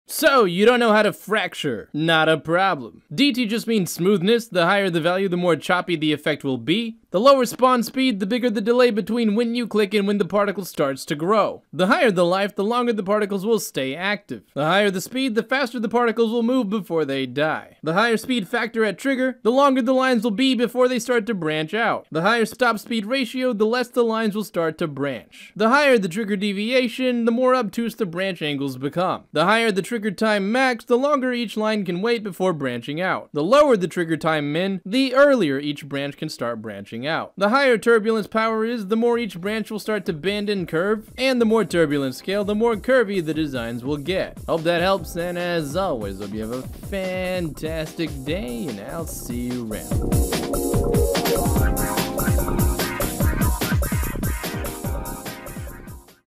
Substance-Painter: Particle Brushes (Fracture!)
Here's how the "Fracture" brush works in 60 seconds!!